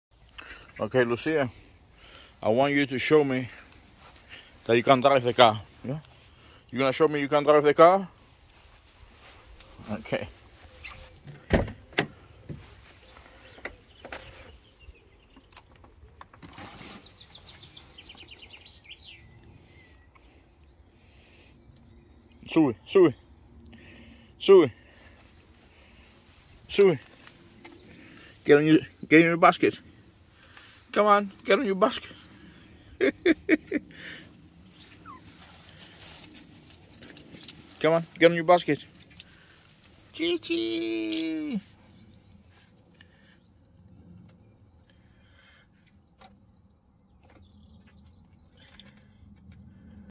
Lucia Maria the Chihuahua wants to drive the car.
Lucia Maria the Chihuahua wants to drive the car.  I think she would if she could, what do you think?